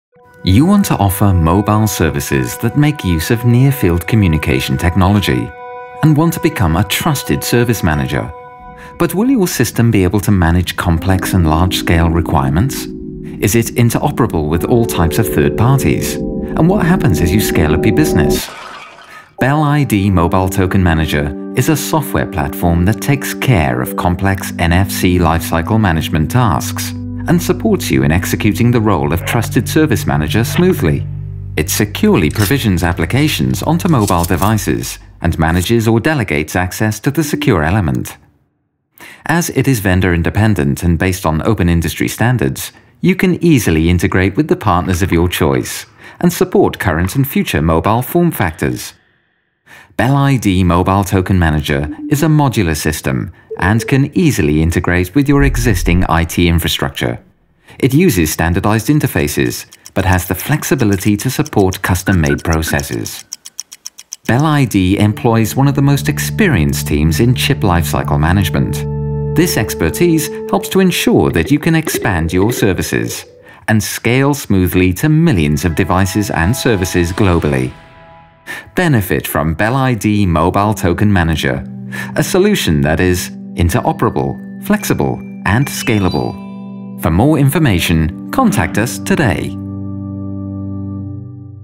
Becoming a Trusted Service Manager?
Bell ID® Trusted Service Manager - a mobile (NFC) lifecycle management system for executing Trusted Service Manager services and for provisioning applications onto mobile handsets.